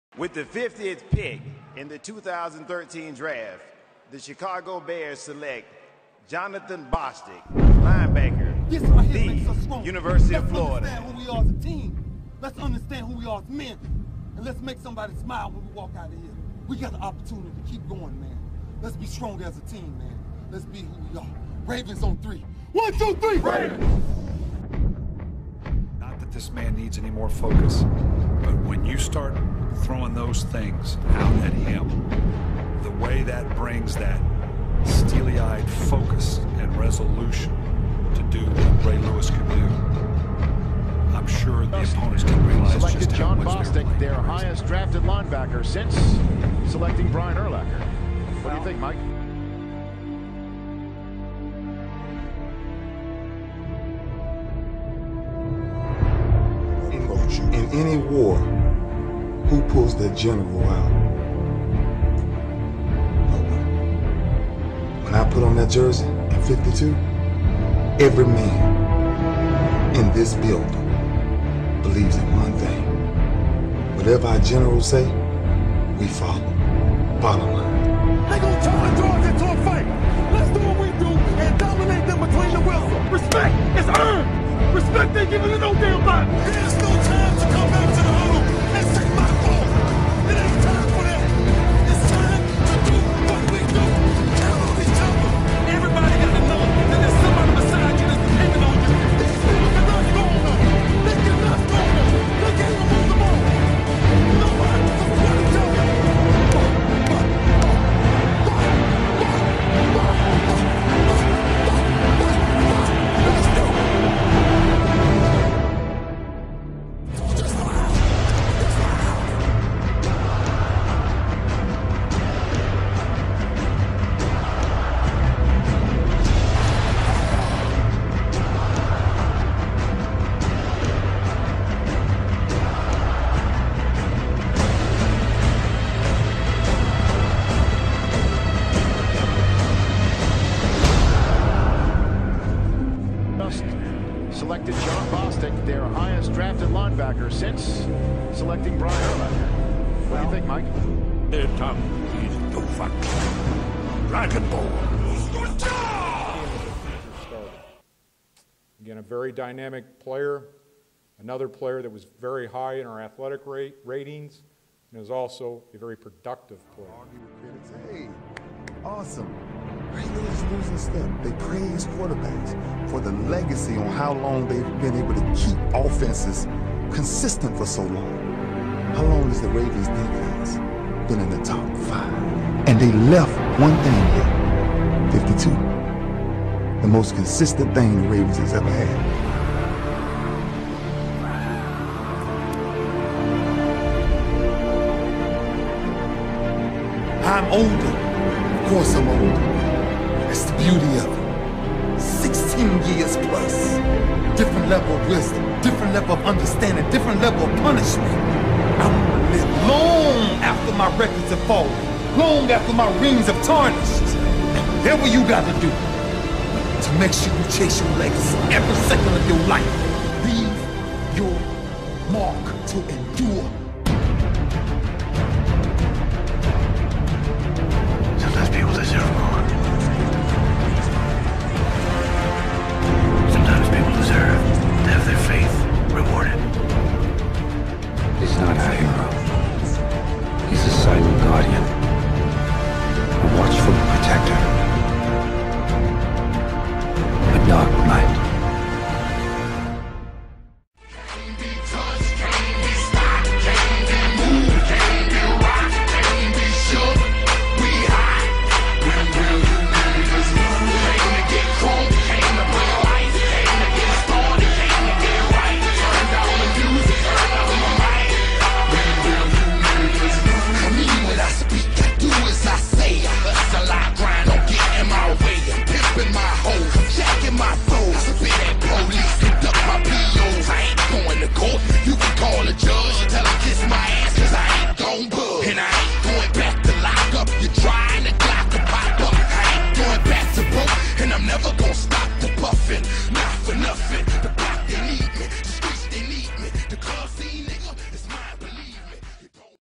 Jon Bostic Highlights | R I S E
A Legend in the making. Chicago Bears newly acquired linebacker Jon Bostic.

By:RemixN007